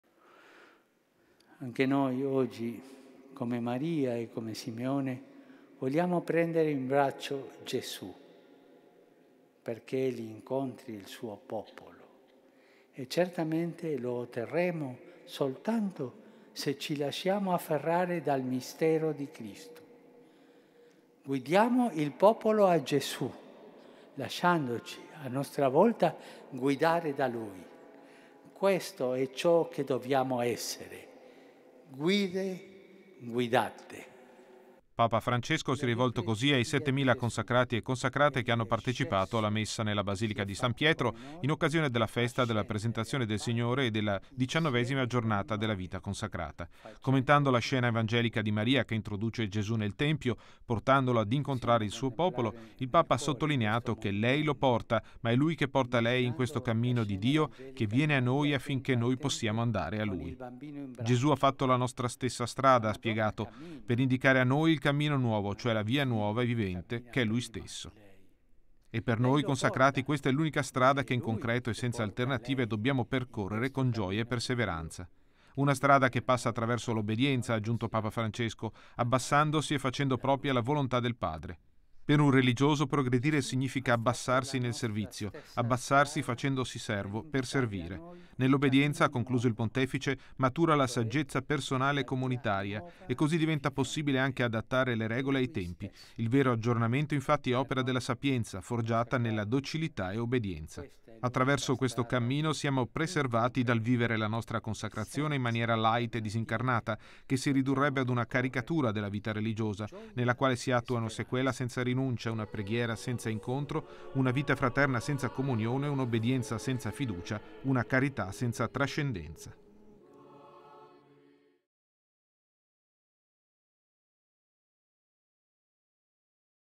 Messa Festa Presentazione di Gesù
IConsacrati, guide guidate da Gesù

Papa Francesco, nella Messa per la Giornata mondiale della vita consacrata, ha ricordato ai consacrati la strada dell’obbedienza e della saggezza indicata da Cristo, da percorrere “con gioia e perseveranza”.